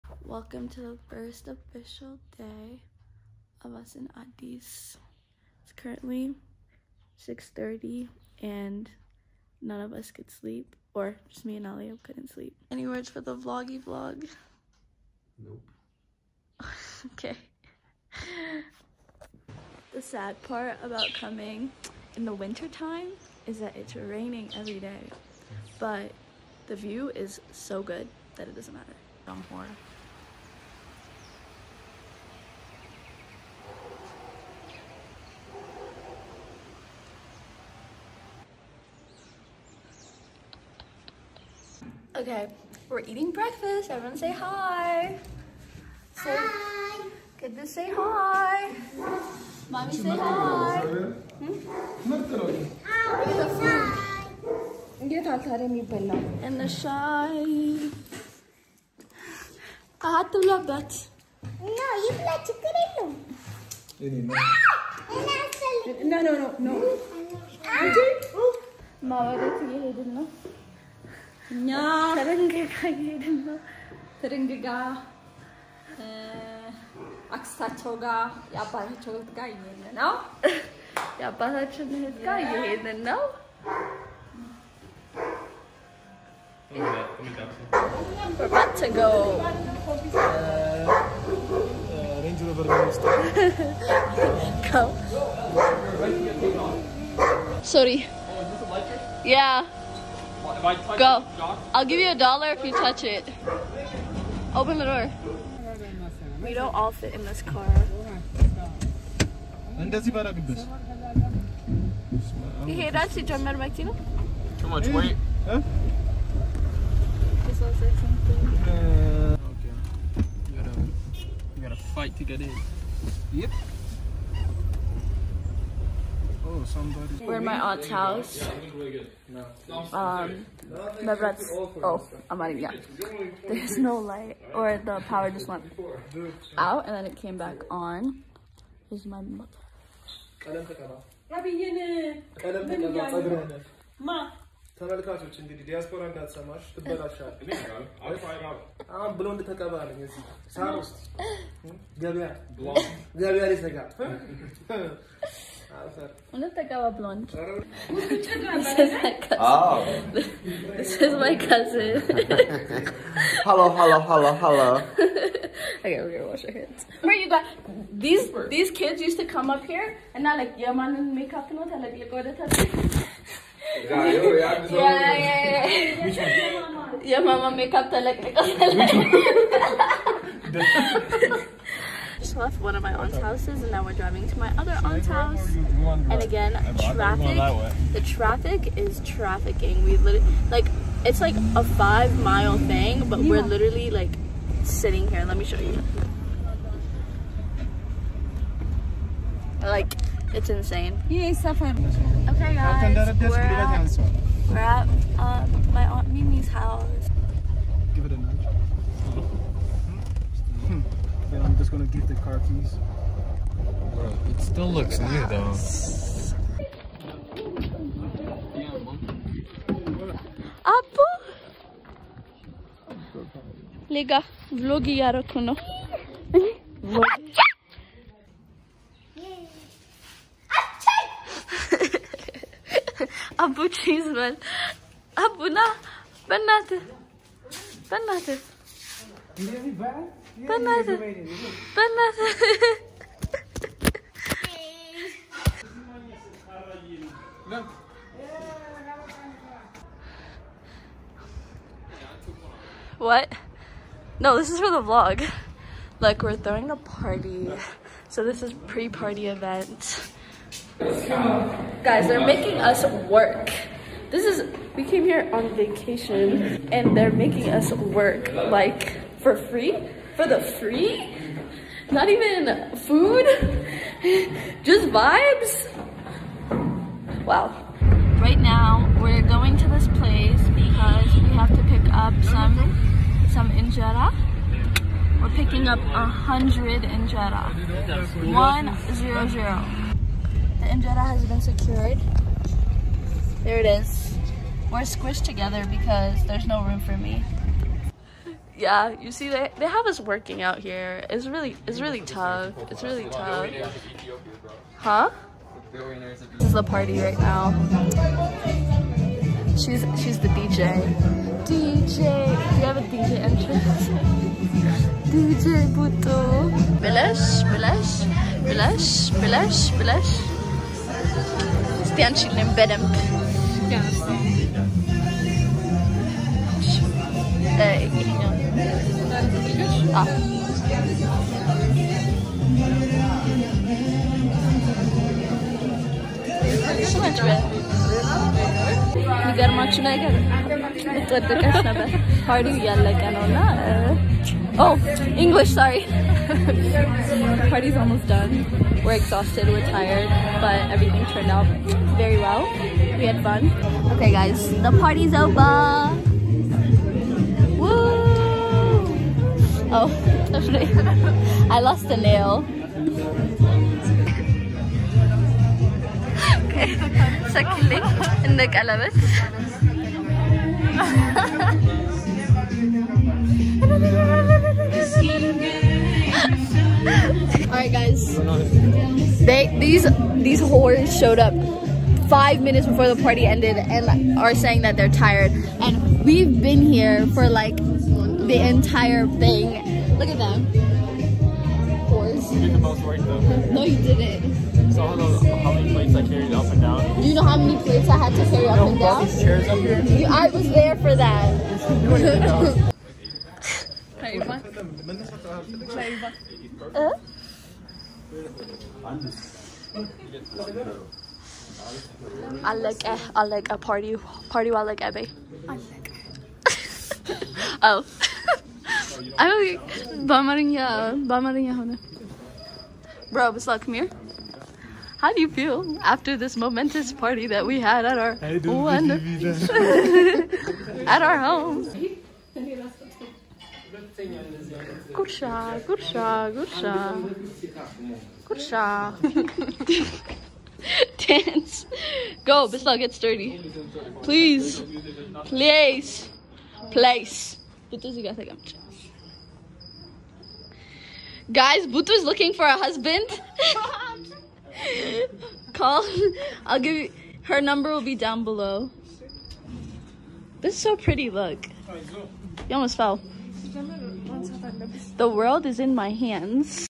ADDIS ABABA, ETHIOPIA VLOG ⏐ Throwing a party
Enjoying the first day of our trip to Ethiopia and throwing a Mels